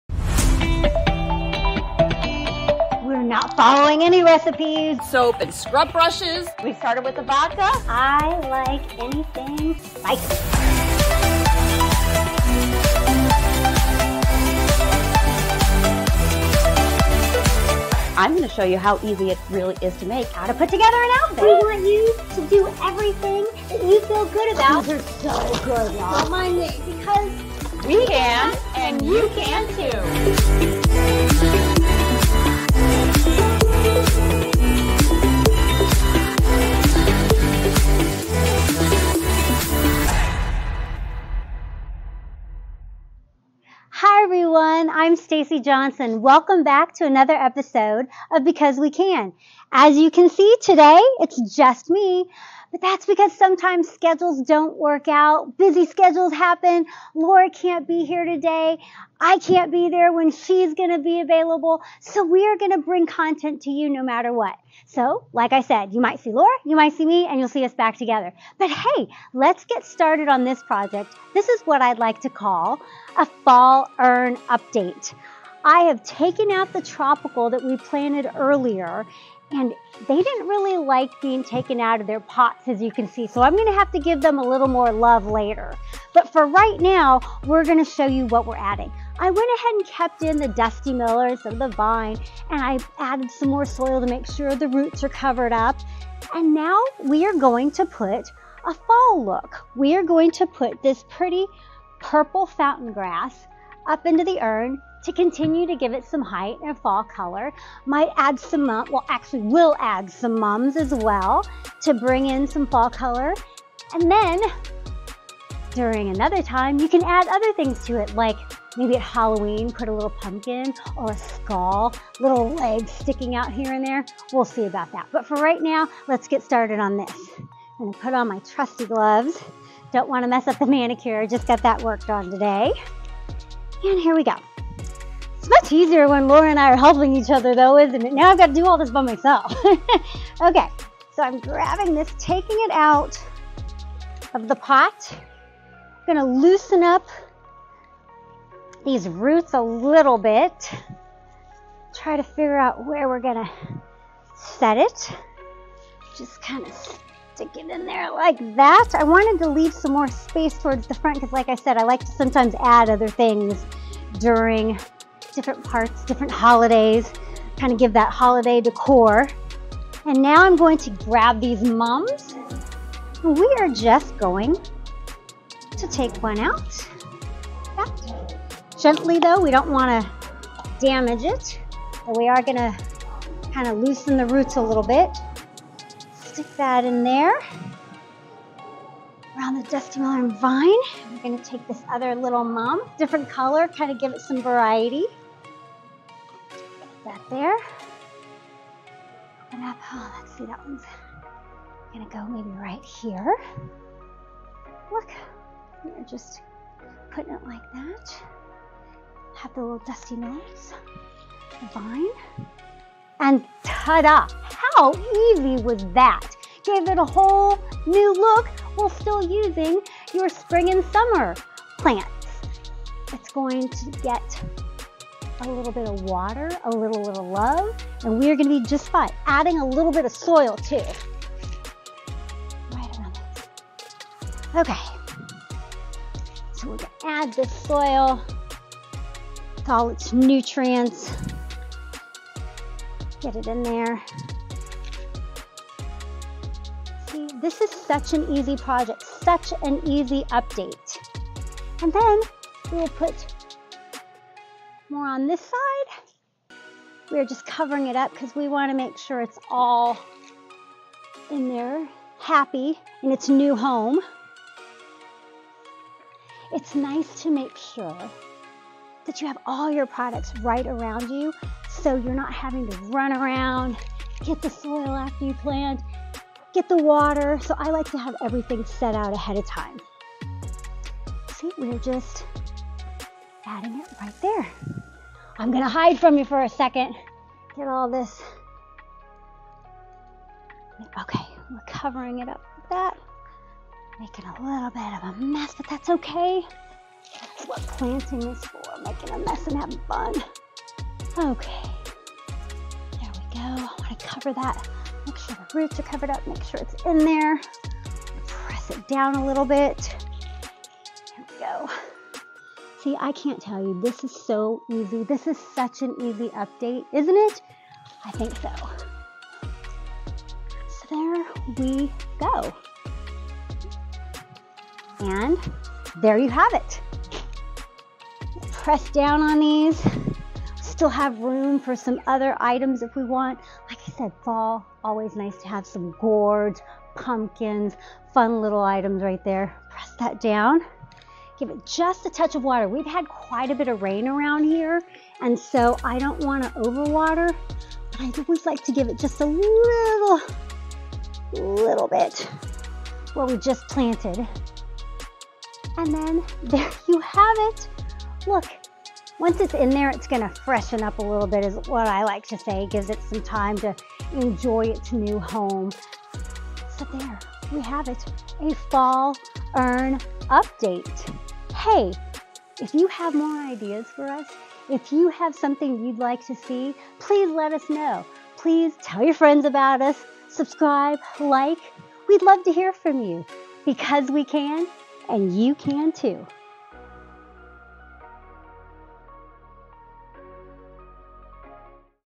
"Fall" in Love With This Urn Update
Transitioning from Summer to Fall planters doesn’t have to be time consuming or expensive. Because We Can co-host, Stacy Johnson, will show you how to make easy updates to the urn from our “Summer Planting” episode, turning it into “Fall Planting”…two simple swaps and you have a new look!
For this project, the Black Canna and Snapdragon were removed and a small amount of fresh potting soil was added to protect the roots of the remaining “summer” plants. Dusty Miller and Green Vinca Vine blend beautifully with the Fall colors of a newly added Purple Fountain Grass, along with Mums in tones of red and yellow. Sprinkle with fresh potting soil, water, and you’re done. Leave room for your festive Halloween and Thanksgiving extras like gourds and pumpkins. Use these small decorative additions to make it your own! 🍂🍁🍃